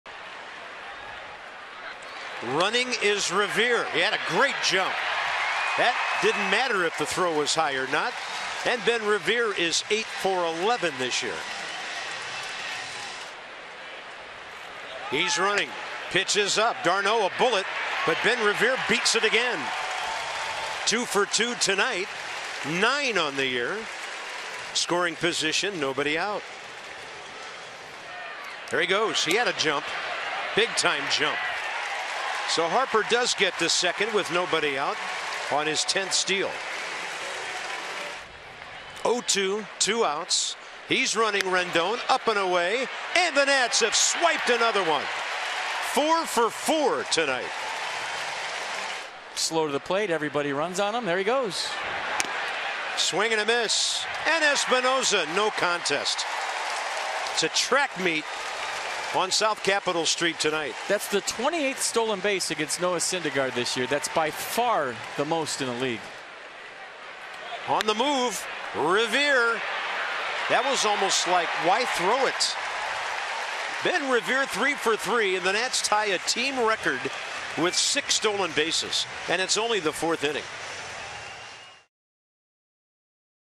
NYM@WSH: Nats steal six bases, tying a Nats record
The Nationals steal six bases to tie a Nationals record of steals in a game, highlighted by Ben Revere's three swipes